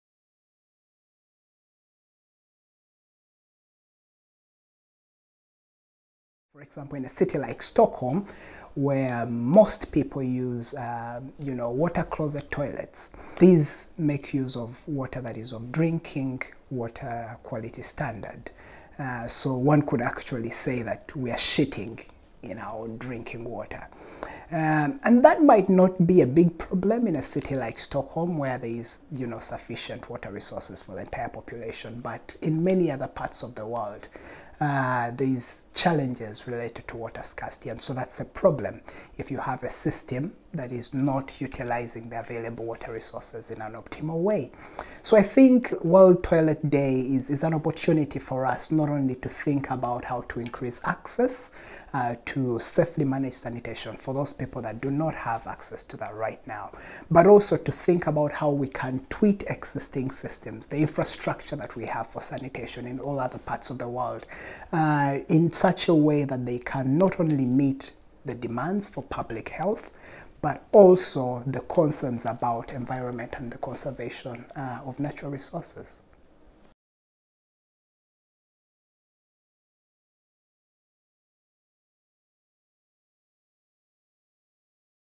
World Toilet Day with Daniel Ddiba
On World Toilet Day 2019, SEI Research Associate Daniel Ddiba talks about the need to consider sanitation from the angle of public health, but also as a natural resource issue.